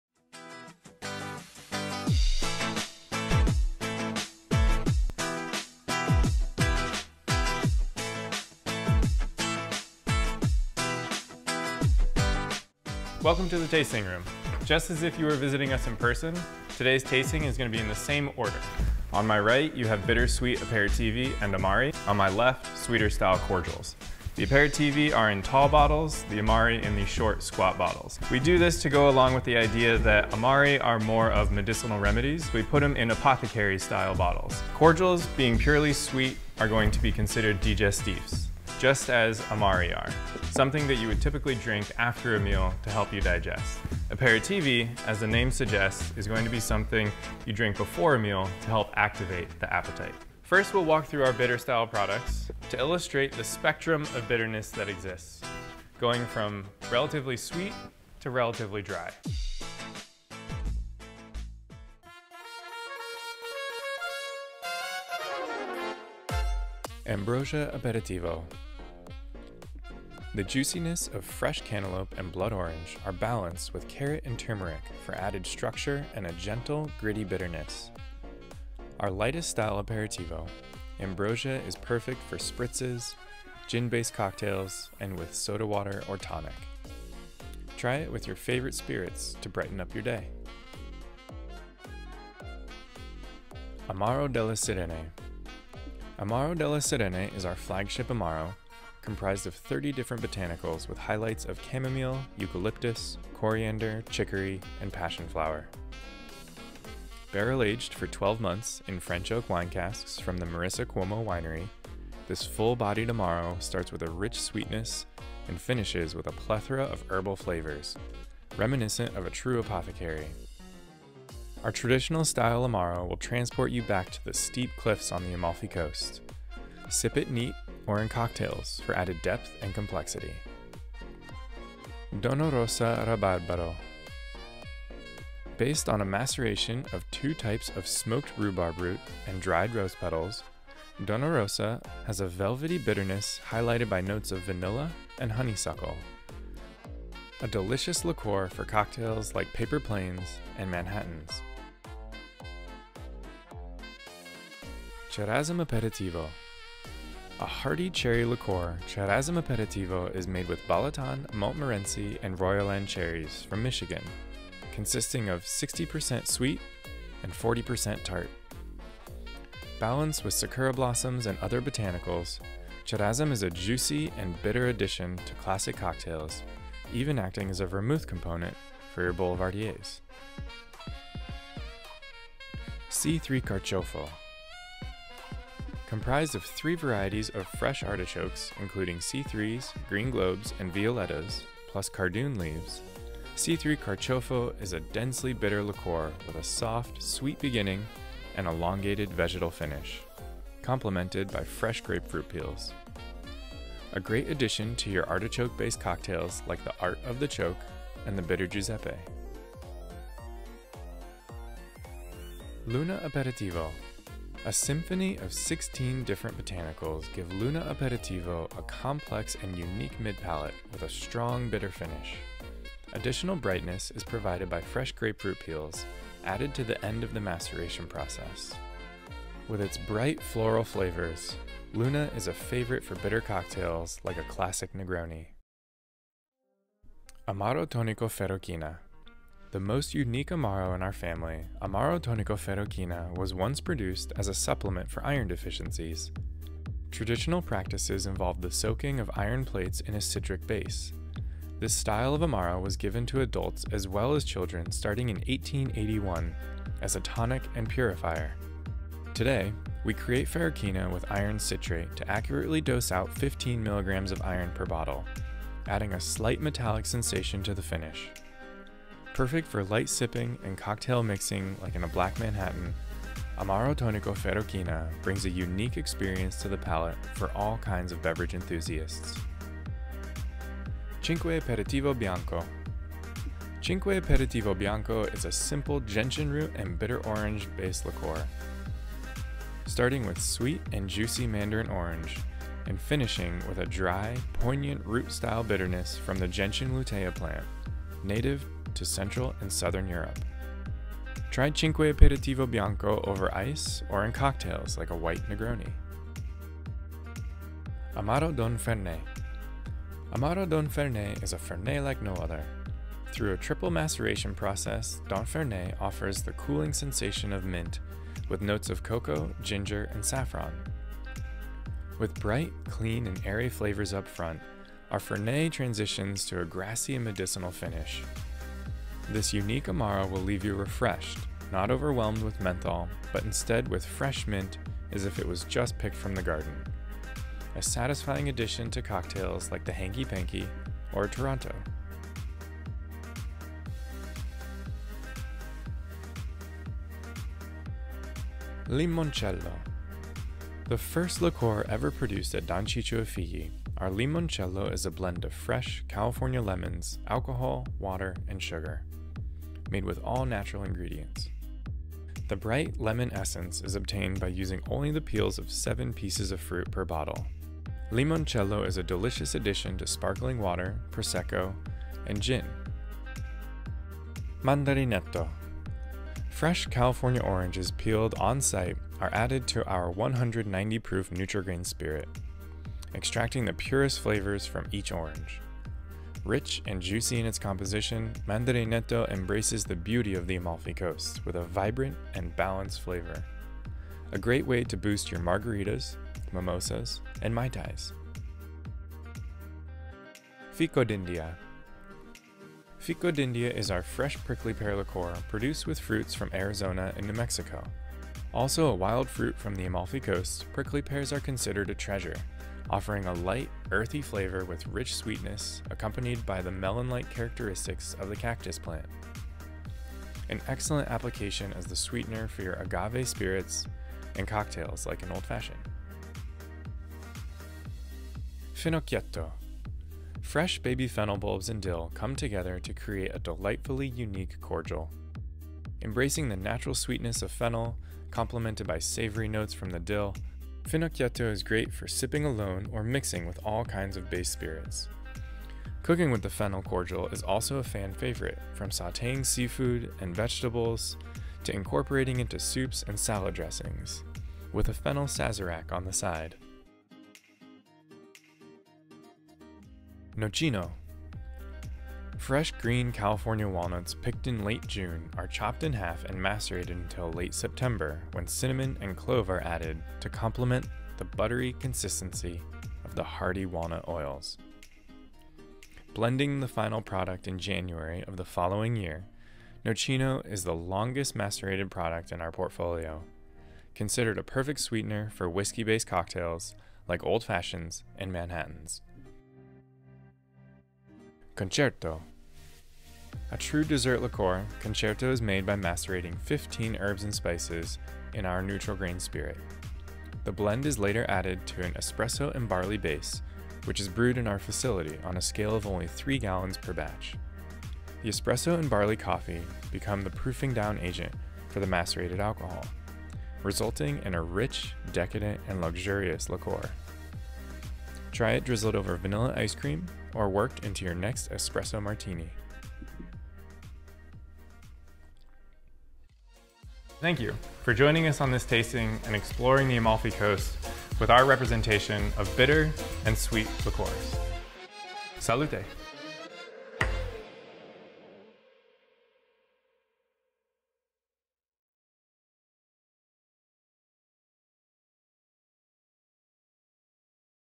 Don Ciccio & Figli Tasting
Take a virtual tasting of the entire lineup of Italian Liqueurs presented by Don Ciccio & Figli's portfolio manager, Jonathan Fasano.